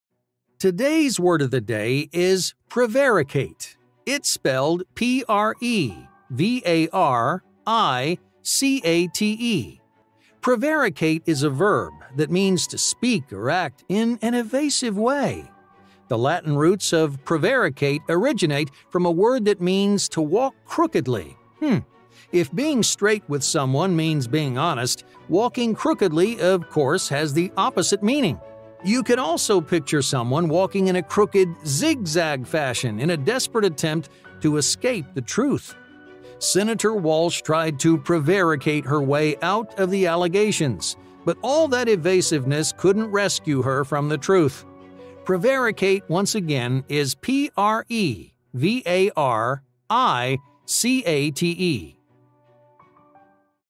Prevaricate (v) to speak or act in an evasive way.
"Alexa, play my Flash Briefing"

Today's word of the day is prevaricate.

It’s spelled P-R-E-V-A-R-I-C-A-T-E.

Prevaricate is a verb that means to speak or act in an evasive way.

The Latin roots of prevaricate originate from a word that means to 'walk crookedly.' If being straight with someone means being honest, walking crookedly, of course, has the opposite meaning. You could also picture someone walking in a crooked zig-zag fashion in a desperate attempt to escape the truth.

Senator Walsh tried to prevaricate her way out of the allegations. But all that evasiveness couldn't rescue her from the truth.

Prevaricate, once again, is P-R-E-V-A-R-I-C-A-T-E.